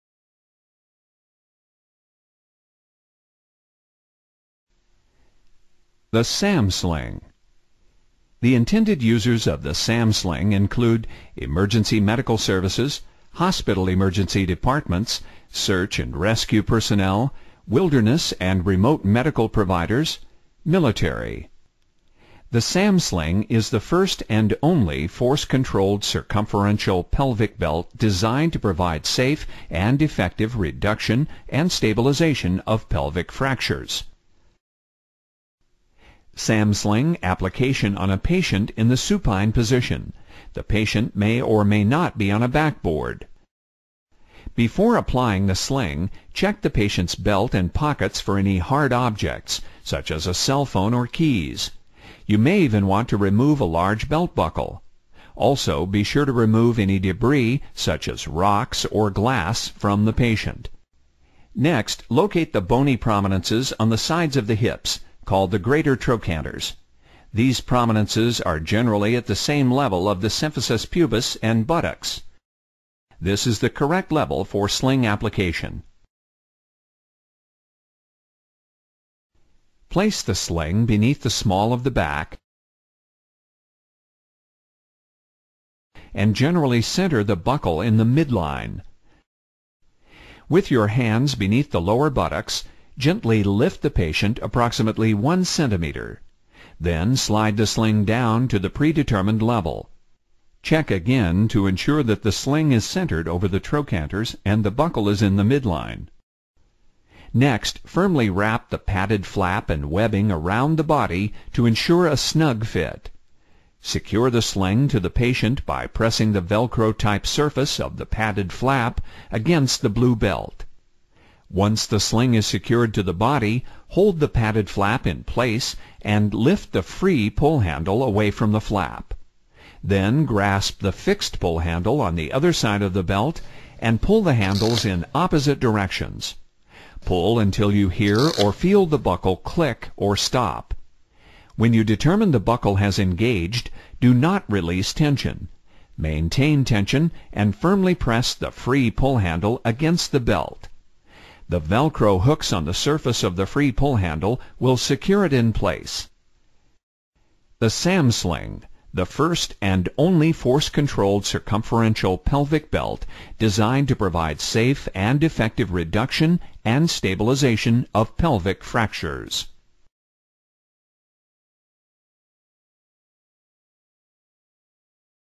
SAM Pelvic Sling Supine Position Application wmv
The SAM Pelvic Sling II is the only force controlled circumferential pelvic belt scientifically proven in peer reviewed studies to safely and effectively reduce and stabilise open-book pelvic ring fractures.